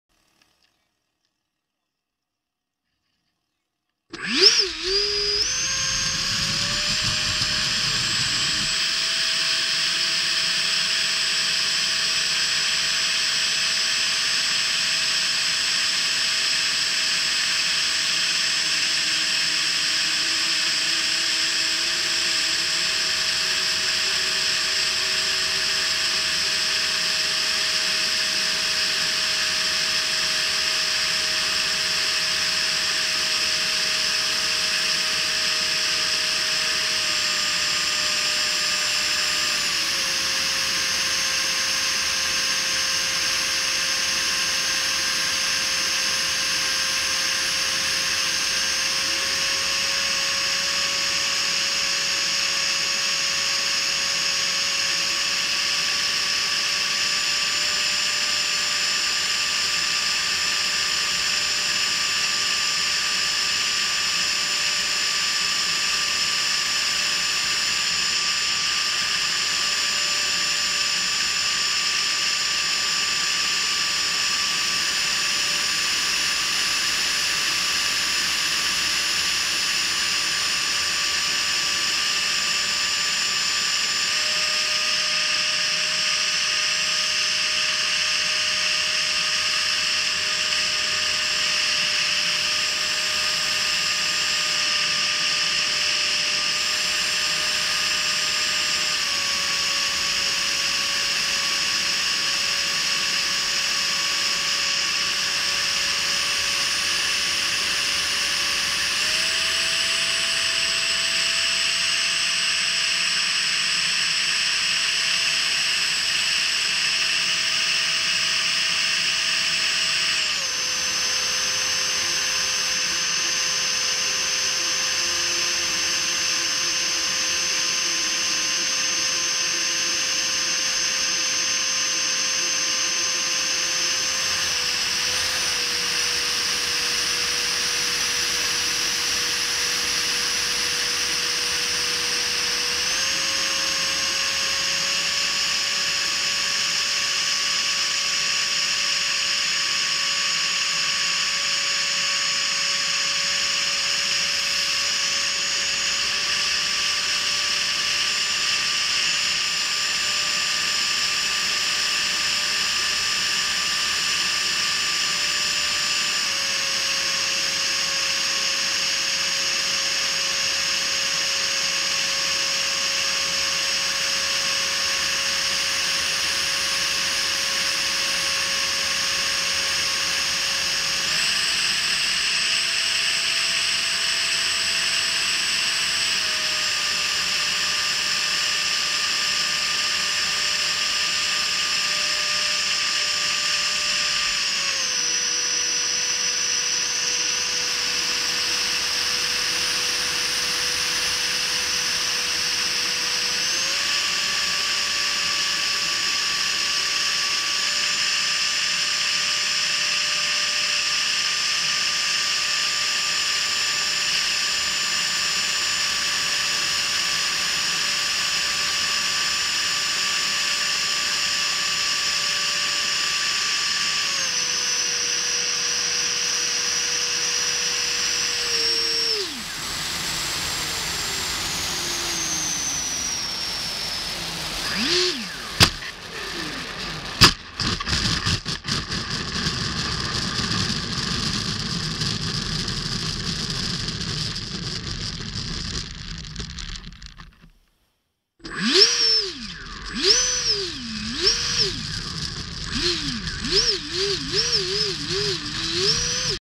Freewing F-16 70mm camo flight #34
Flying site @ Cyberjaya Malaysia
Edf - new freewing edf inrunner 70mm
Battery pack - HRB lipo 6s 5000mah 50c
Camera onboard : HTC Re

Sunny sunday flight 03/03/2019🍻